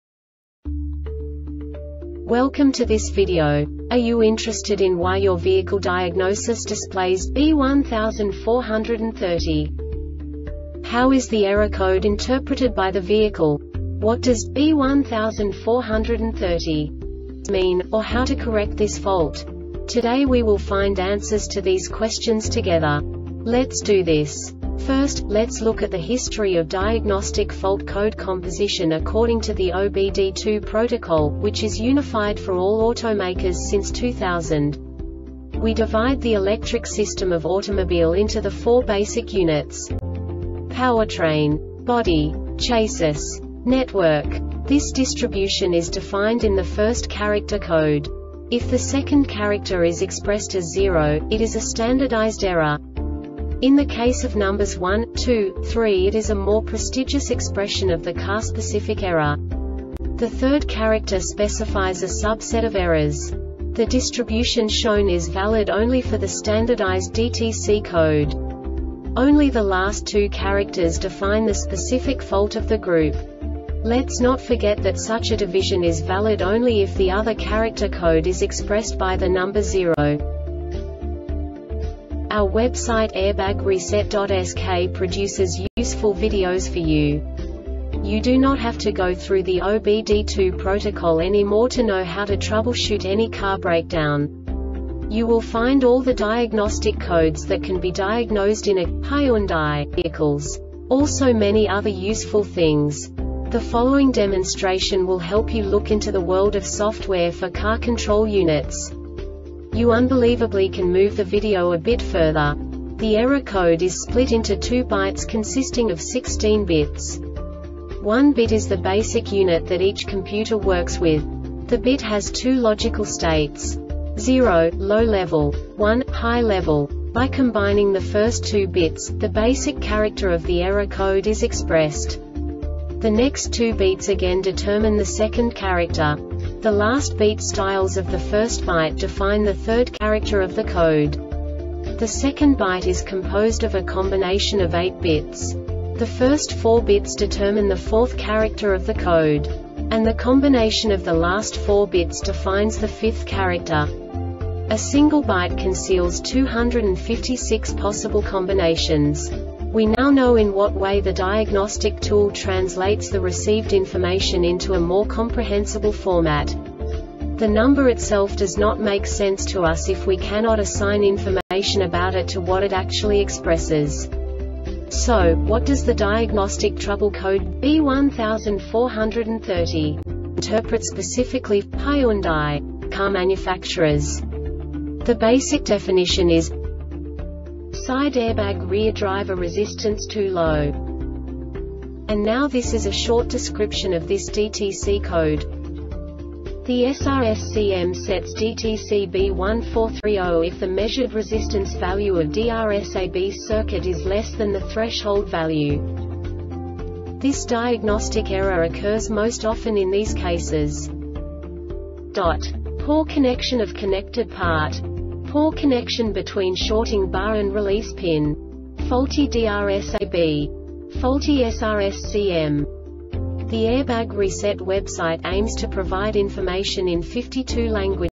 DTC Hyundai B1430 Short Explanation
Contents:
0:21 Basic DTC analysis according to OBD2 protocol standard.
1:48 Insight into programming
2:58 A diagnostically understandable description of the Diagnostic Trouble Code
3:05 Basic error definition

Make:
Hyundai
Code:
B1430
Definition:
Side Airbag Rear-Driver Resistance too Low
Description:
The SRSCM sets DTC B1430 if the measured resistance value of DRSAB circuit is less than the threshold value.
Cause:
Poor connection of connected part.
Poor connection between shorting bar and release pin.
Faulty DRSAB.
Faulty SRSCM.